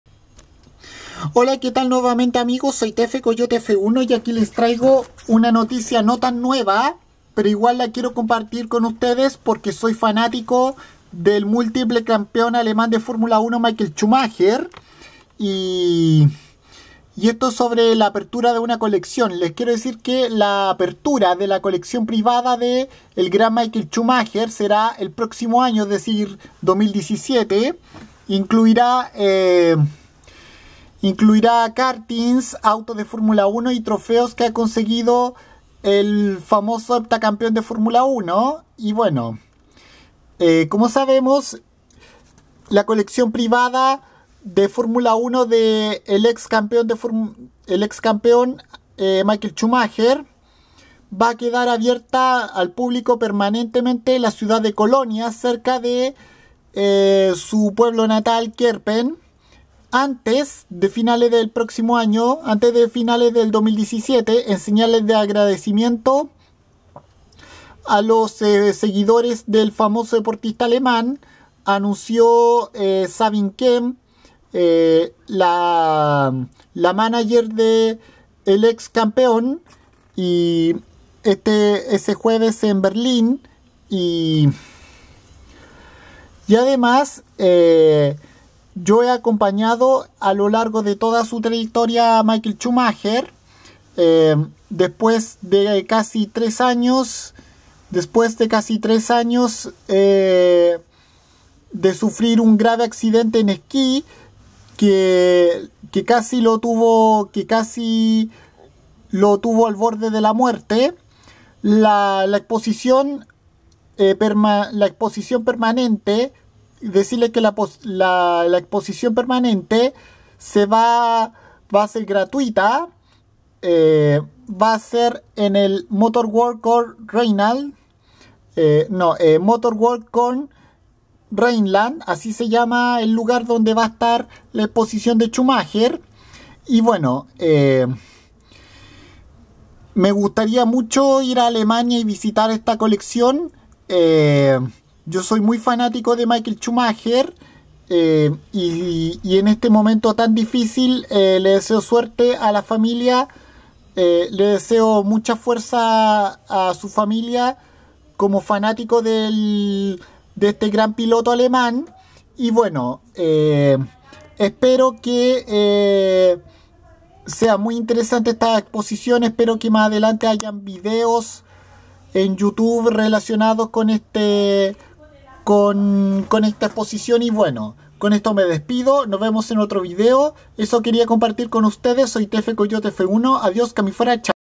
Exposición permanente de Michael Schumacher será gratuita y abrirá en 2017
En Colonia Alemania cercano a su pueblo natal Kerpen se abrirá una exposición permanente que recordará la trayectoria deportiva del heptacampeón alemán de F1 Michael Schumacher que será gratuita y se expondrá por primera vez al público y que tendrá kartings,autos de F1,trofeos,etc. que ha recopilado el ex piloto alemán quien a casi 3 años de su terrible accidente en esquí esta luchando por su vida en estos momentos